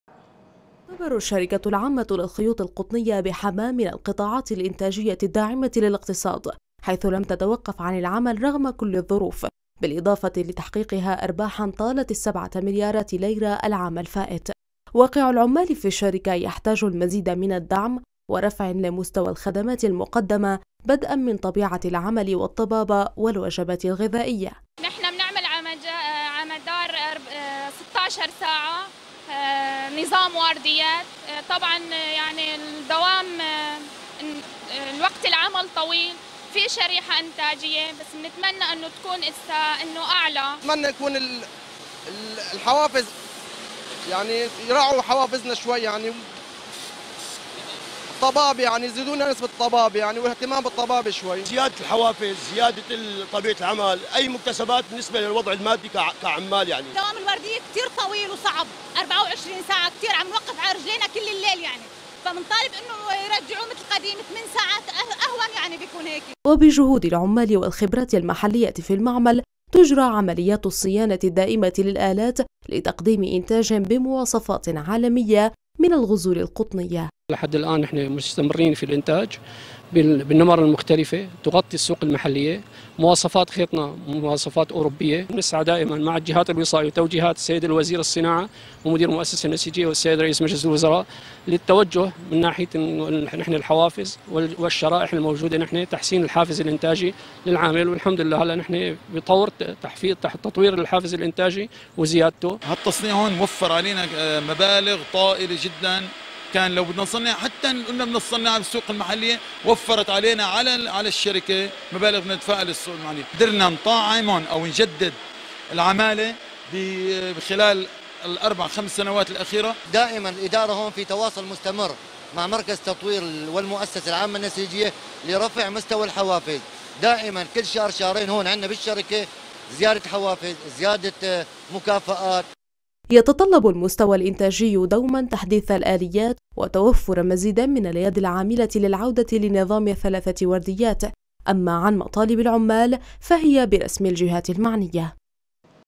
الشركة العامة للخيوط القطنية بحماة
أرباح قدرت بـ 7 مليارات ليرة في 2021 .. والعمال بحاجة مزيد من الدعم
تقرير : عبد الغني جاروخ - قراءة : رهام علي
تصوير: محمد ضبع - مونتاج : رضوان أبو شامة